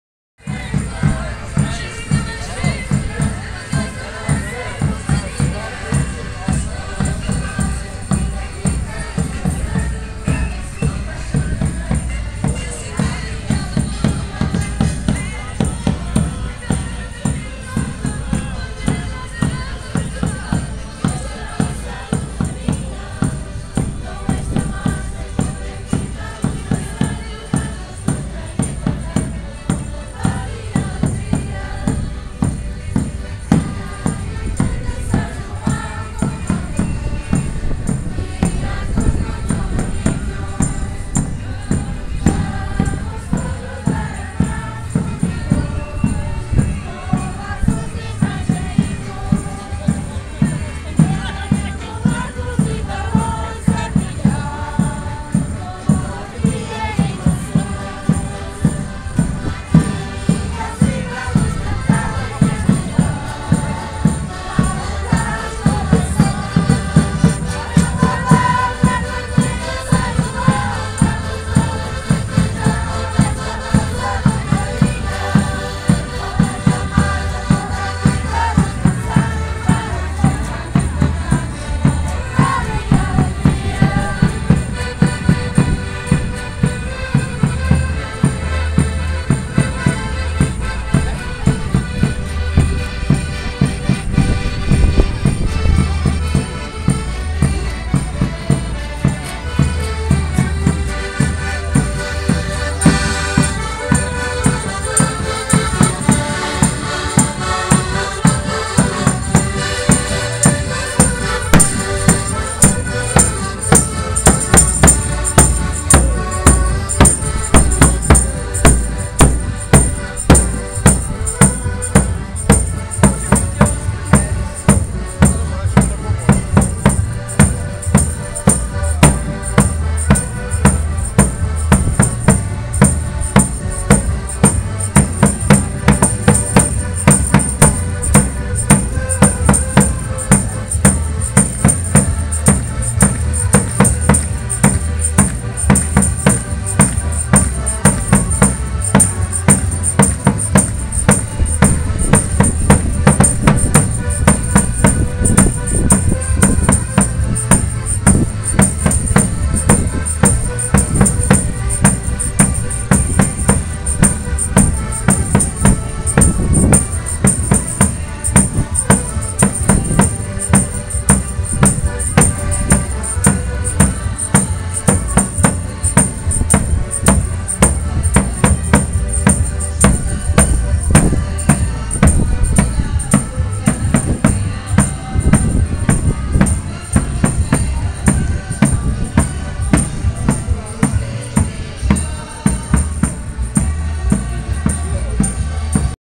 Madeira Marchas Populares - Grupo Casa do Povo Ribeira Brava de São João em São Martinho do Funchal
Grupo da Casa do Povo da Ribeira Brava nas Marchas de São João em São Martinho do Funchal Madeira Portugal